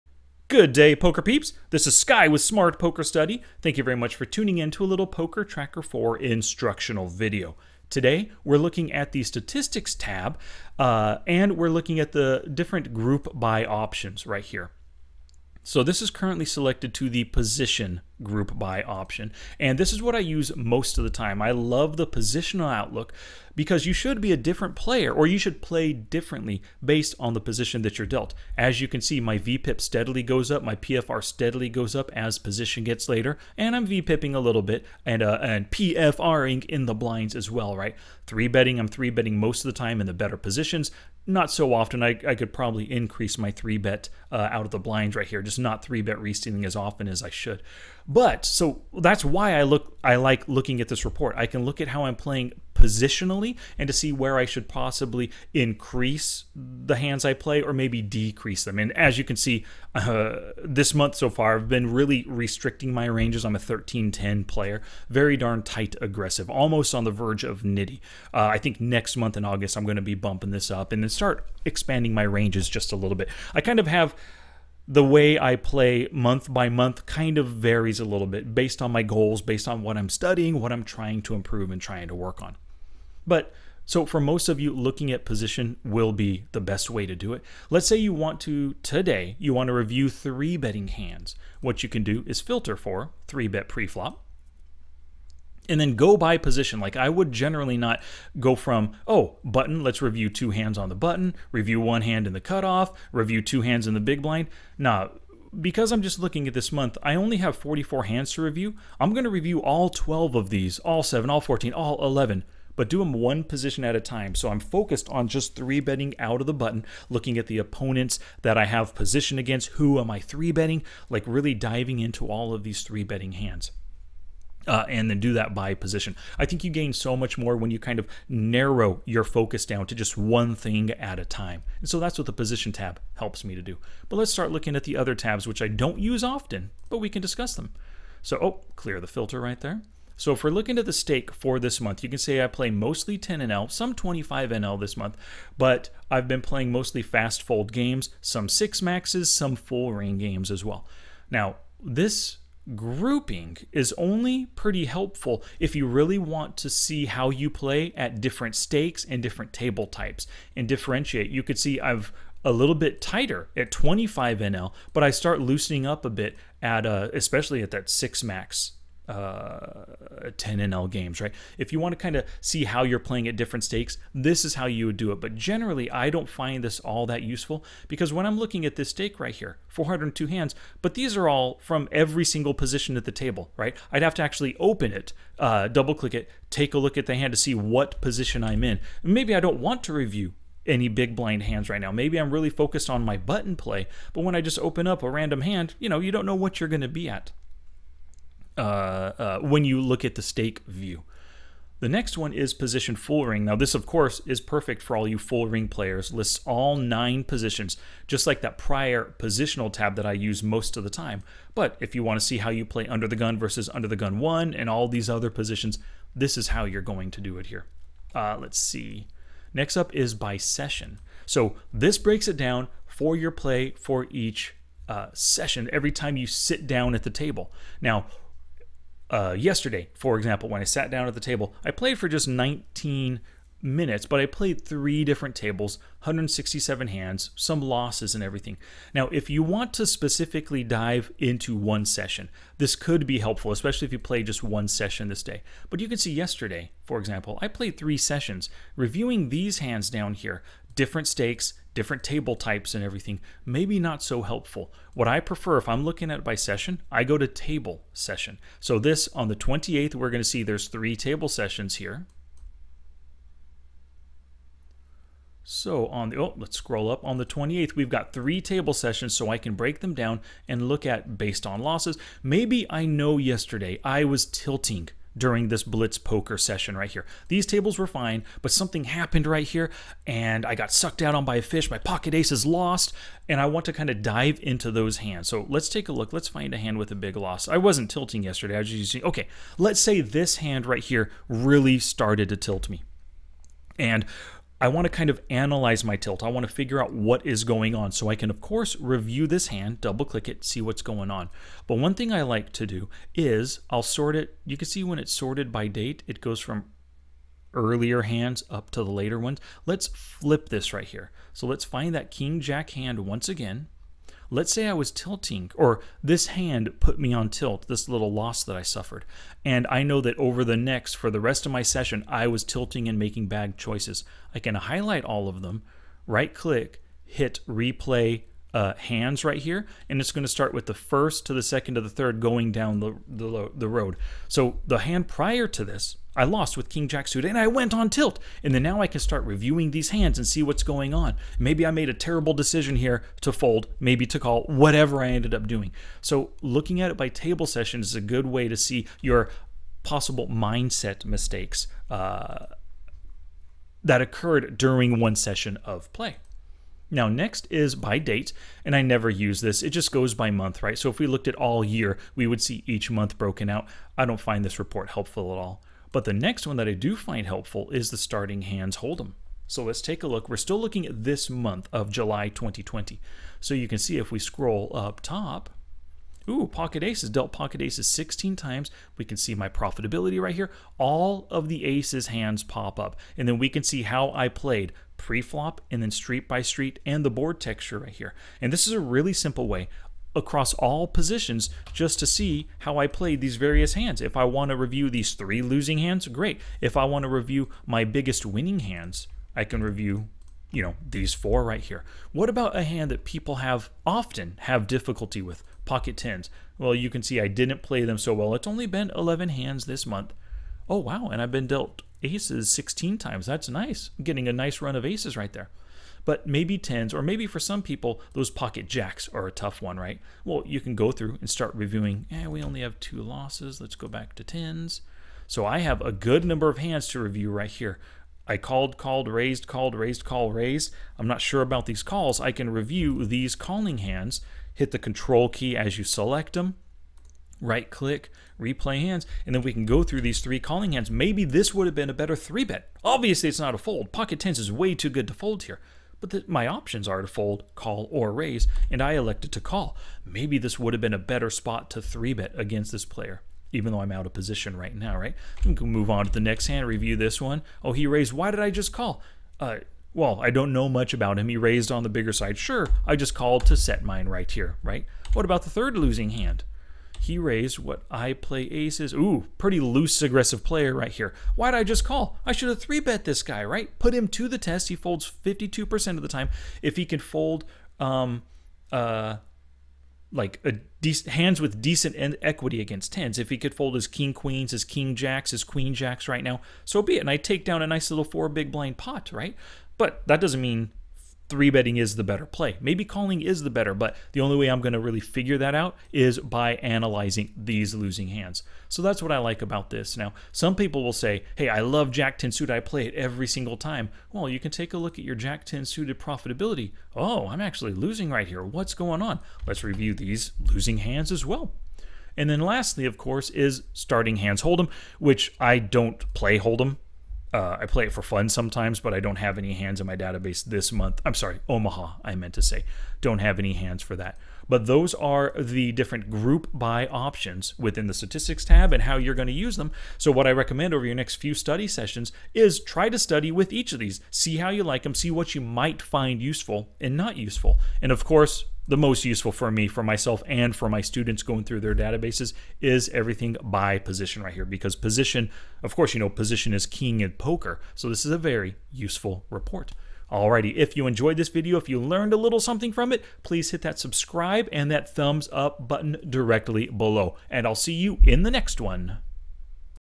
Choose the most relevant statistics report in PokerTracker 4
I show you each of the "group by" options on the PokerTracker 4 statistics tab. They're each used for different types of study and I discuss what each report is best for.